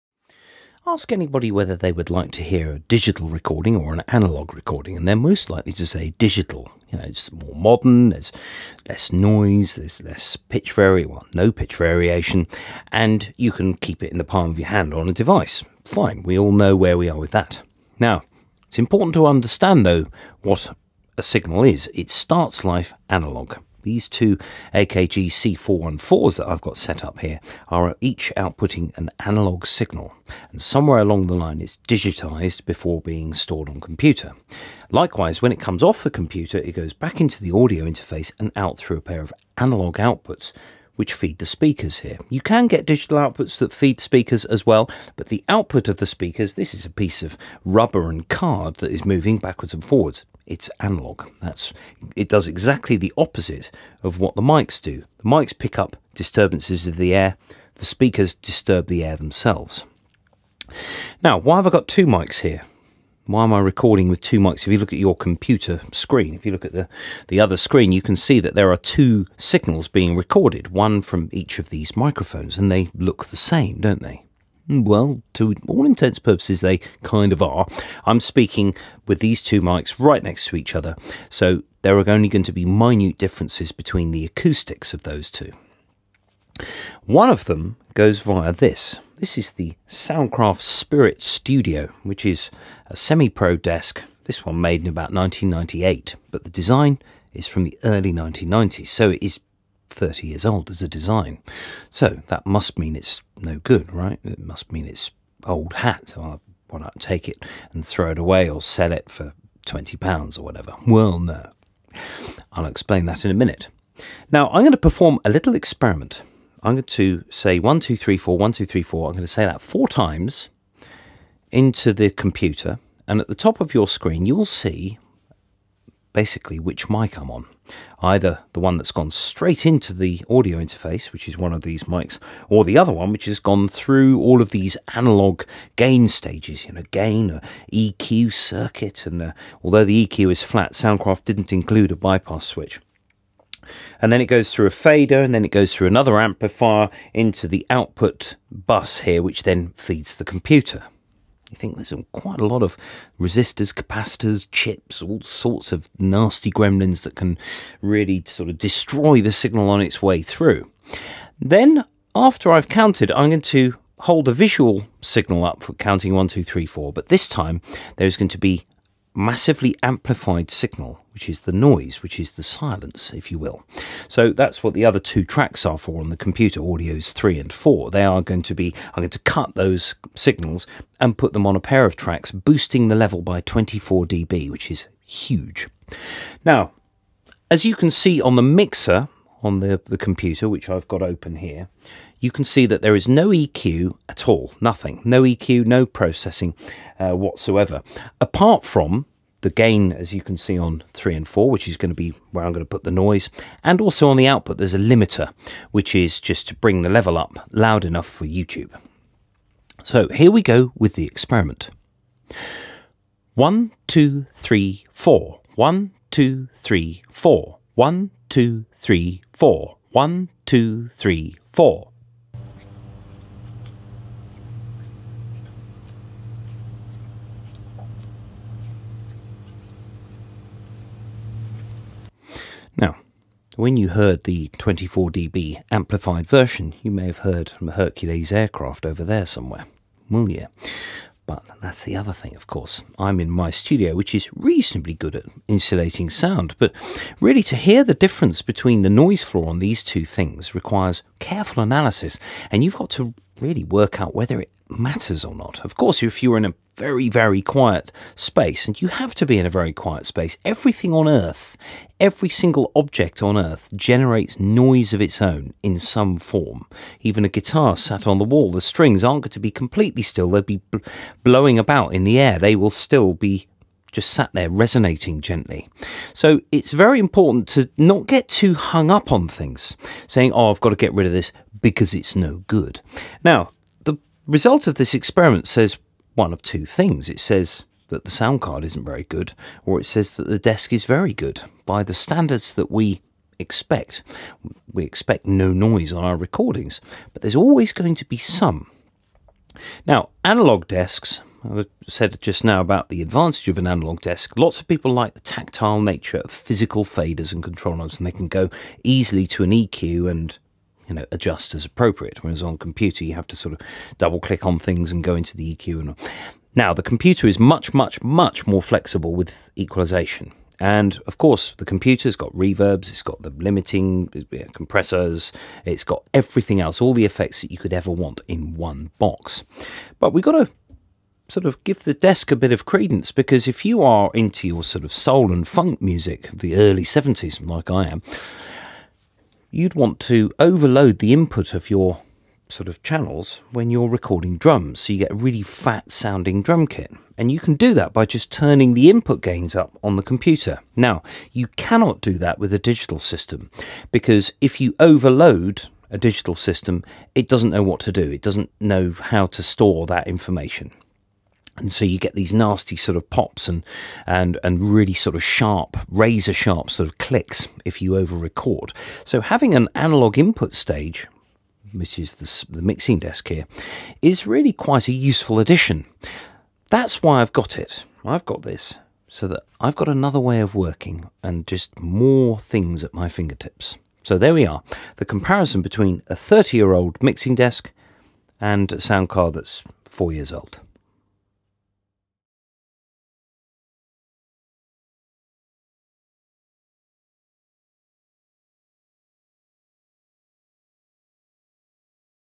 Analogue versus Digital Part 1: Does an Analogue Mixer Still Cut it in the Computer Age?
So, for part 1.

Before you throw old (but fully working) bits of kit away, think about what it does, and whether it's really a laughing stock in the modern studio (not that you should care)...

Most people glaze over when reading the technical specifications of a piece of equipment. Though they do give you an idea, it's always best to use your ears.

Here, I compare a 1998 Soundcraft Spirit Studio analogue mixing desk with a 2017 Focusrite Saffire Pro 56 audio interface. I run two identical mics (stereo matched pair of AKG C414):

1. One mic going through the analogue stages (mic pre - a buffer amplifier into EQ section (set flat) - through another buffer amplifier - through a fader - along a buss - into another buffer amplifier through a sub-mix output and into the Focusrite on a line input, and:

2. One mic going straight into a mic pre of the audio interface, so a far more direct route, and very quickly digitised.

I then compare the noise floor of both by boosting ambience by 24dB, for clarity and ease of comparison...

In the next video, I compare "straight to digital" with an open reel analogue tape recorder - you can see it here: